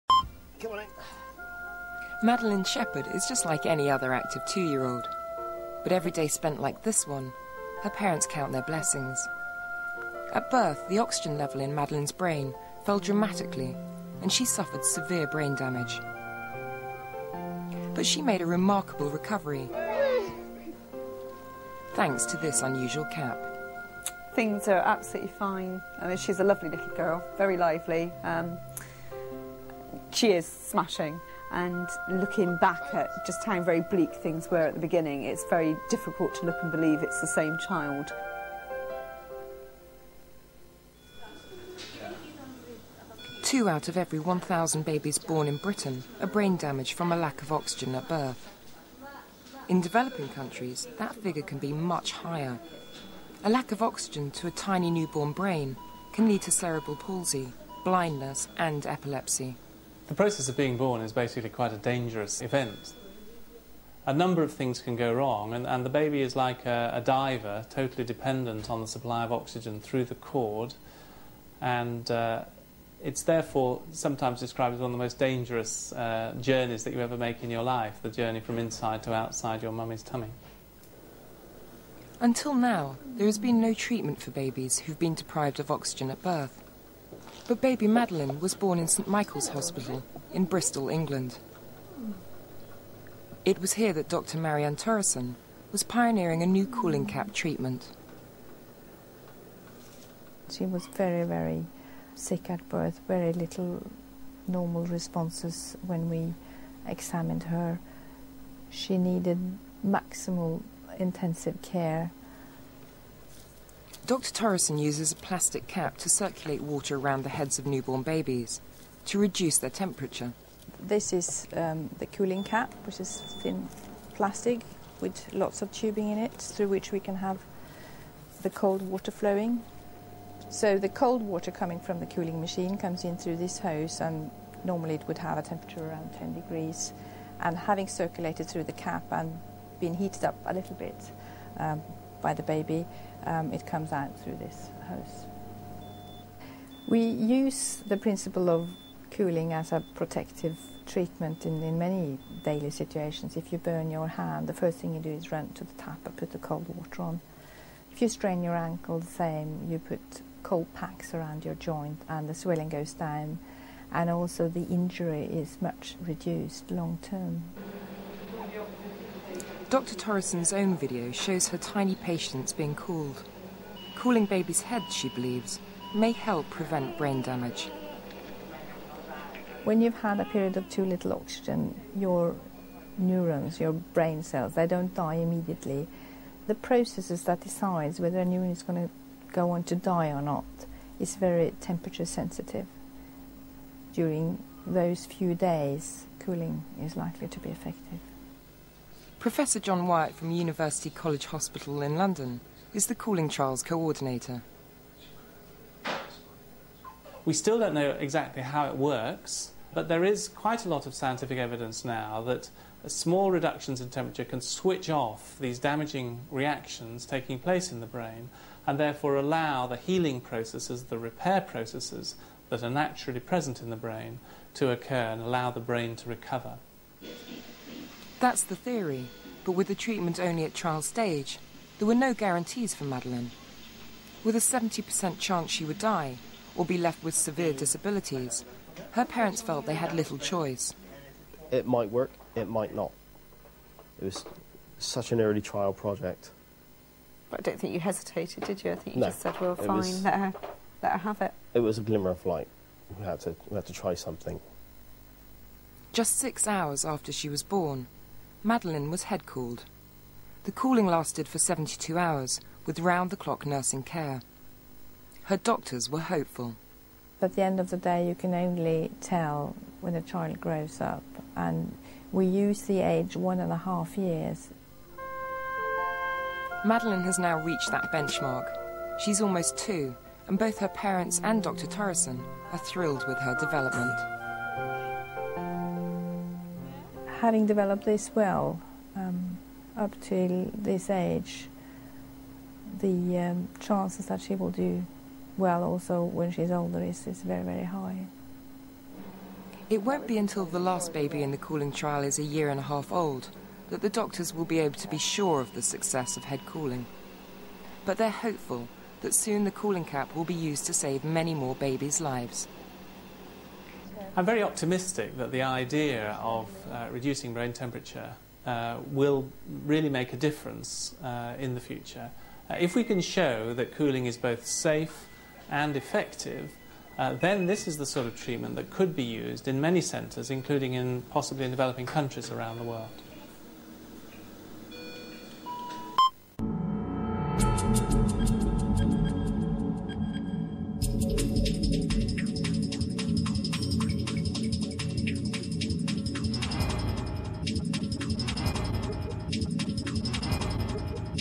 Cool Baby - UK
1 September 2001
Cooling has been used in medicine for centuries. Now doctors in England are taking this concept a stage further -- with a cap to cool babies who are in danger of brain damage from lack of oxygen at birth.